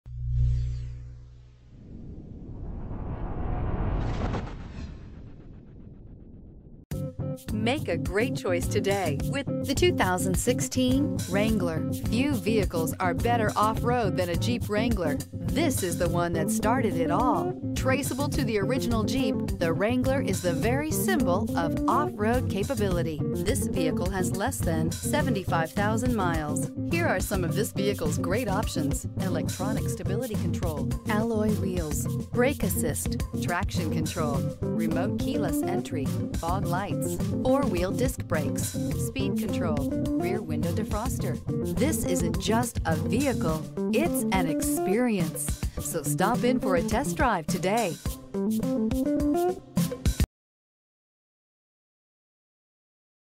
2016 Jeep Wrangler P26228
2016 Jeep Wrangler Unlimited Sahara

4D Sport Utility Billet Silver Metallic Clearcoat 16/20 City/Highway MPG 2016 Jeep Wrangler Unlimited Sahara 4WD 5-Speed Automatic 3.6L V6 24V VVT Wrangler Unlimited Sahara, 4D Sport Utility, 3.6L V6 24V VVT, 5-Speed Automatic, 4WD, Billet Silver Metallic Clearcoat, Black Cloth.brbrRecent Arrival!